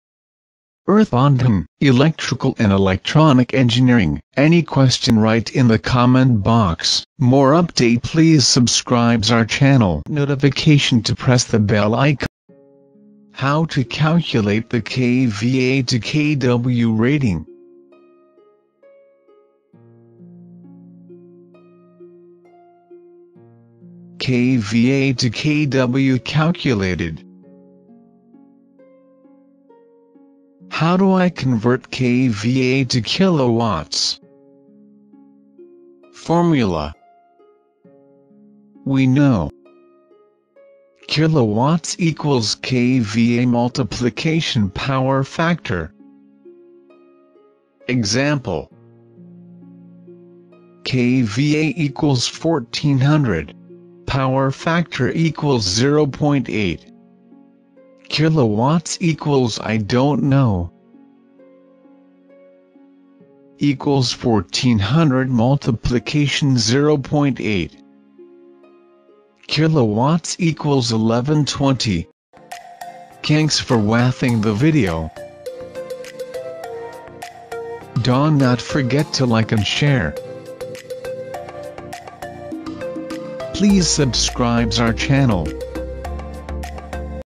How to convert KVA to kw | Earthbondhon
How to convert KVA to KW | KVA To KW | convert kw
In this video, we explain KVA to KW conversion. we convert power to active power, apparent power is the total power that is coming and active power is the power which power we use.